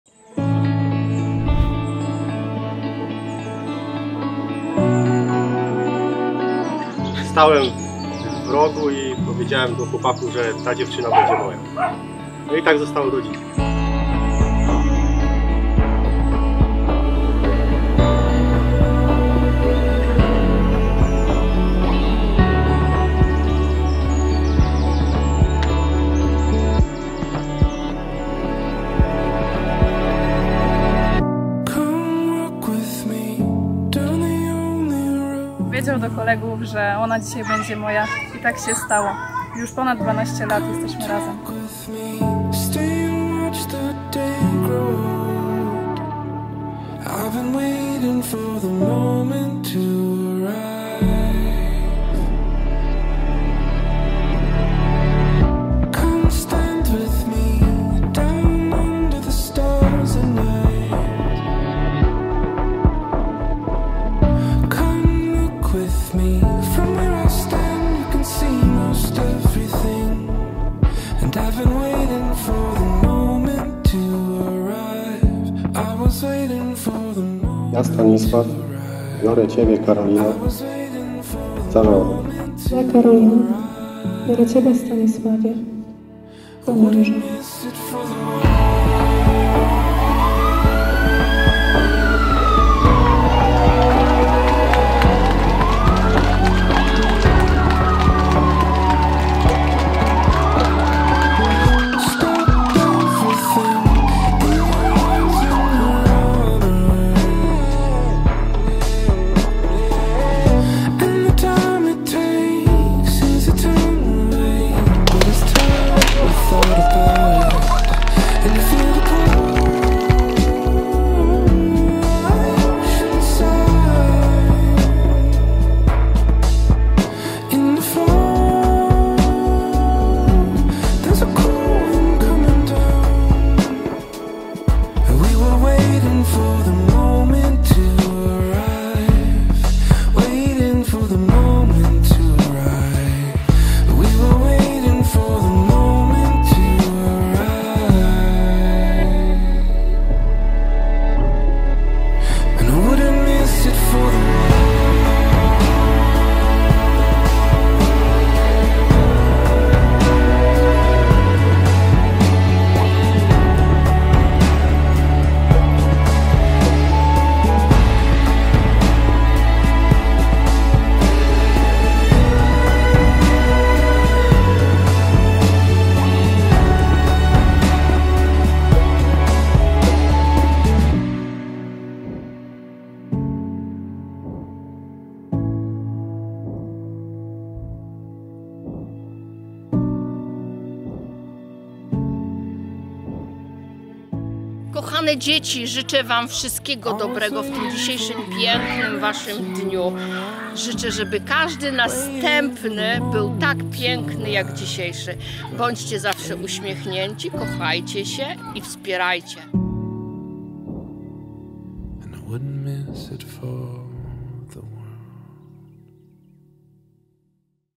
Teledysk Ślubny - Karolina & Stanisław [4K]
Prezentujemy Państwu teledysk ślubny Karoliny & Stanisława
Wykonanie filmu: HORYZONT Wideofilmowanie
Miejsce ślubu: Kościół - Redło pw. Narodzenia św. Jana Chrzciciela
Miejsce wesela: Sala weselna Helena Park. Górnicki J. - Świdwin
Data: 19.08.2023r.

Jeśli jesteś zainteresowany naszą usługą, odezwij się do nas na facebooku, a przedstawimy Ci naszą ofertę - najlepszą w okolicy!